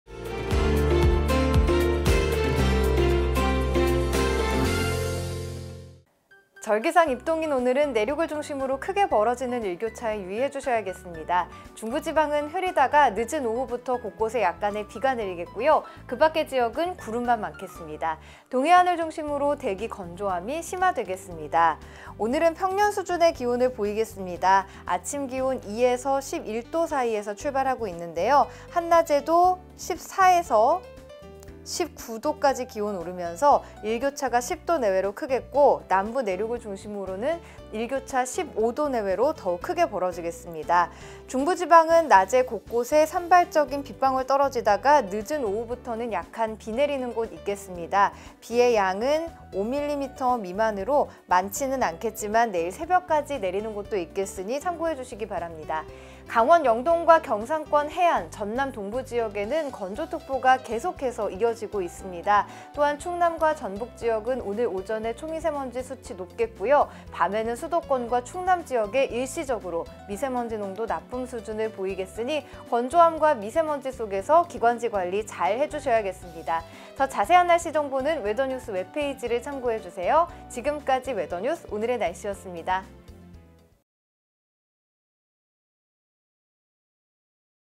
[웨더뉴스] 오늘의 날씨 (11월 07일 07시 기준)
절기상 '입동'인 오늘은 내륙을 중심으로
크게 벌어지는 일교차에 유의해 주셔야겠습니다.

중부지방은 흐리다가 늦은 오후부터 곳곳에 약간의 비가 내리겠고
그 밖의 지역은 구름만 많겠습니다.

동해안을 중심으로 대기 건조함이 더욱 심화되겠습니다.